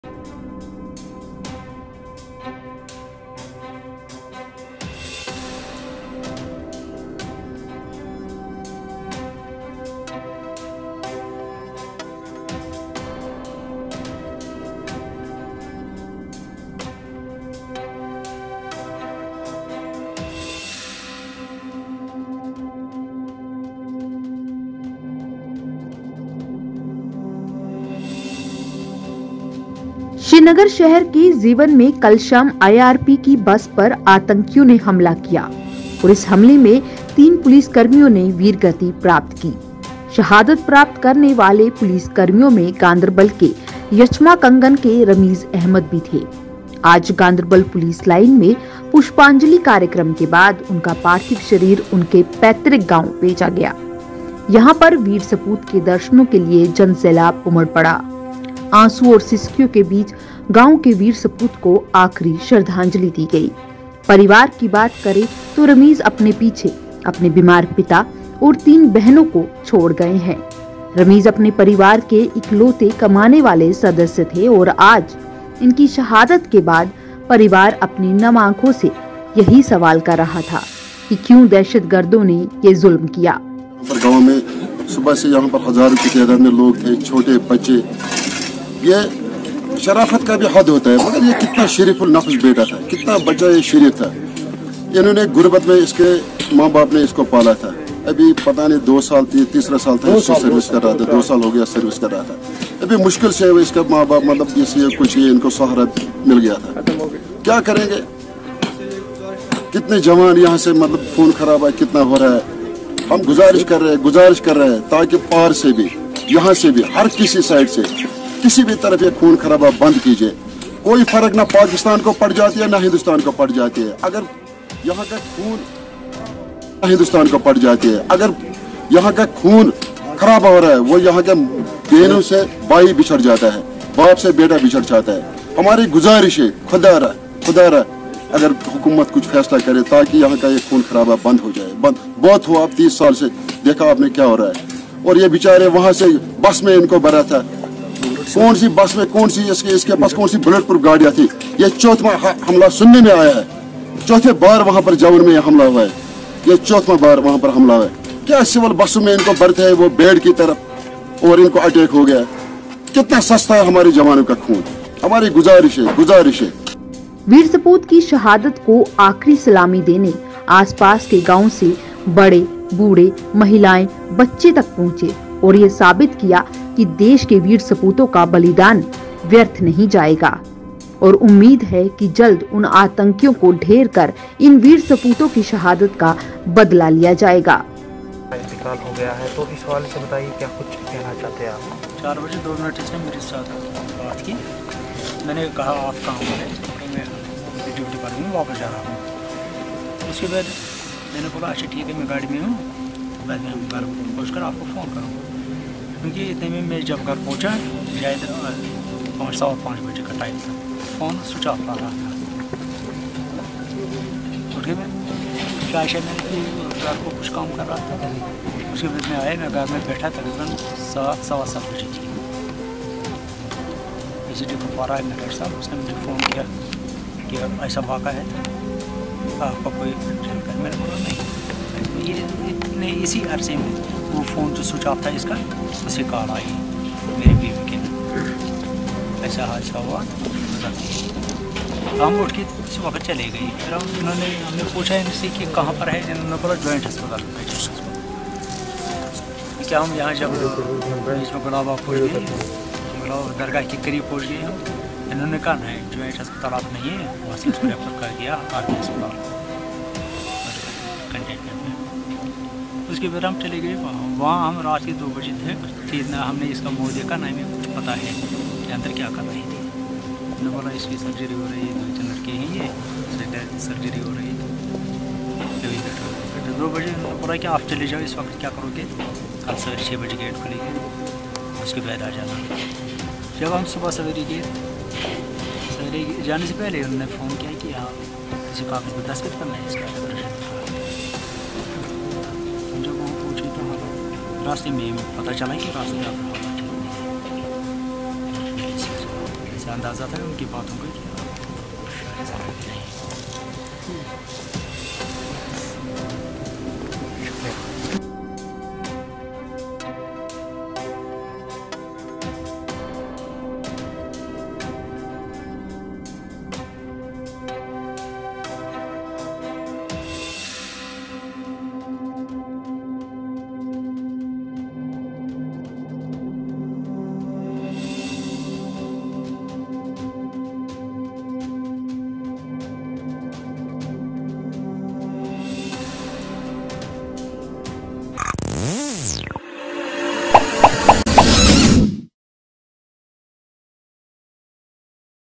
Massive crowd gather at Gqnderbal to pay Tribute to the Martyr in Zewan attack
Massive crowd gather at Gqnderbal to pay Tribute to the Martyr in Zewan attack | Report By | Rayees Shah